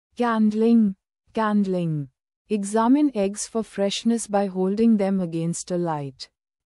'Candling' Meaning and Pronunciation
Word: Candling
Meaning: examine eggs for freshness by holding them against a light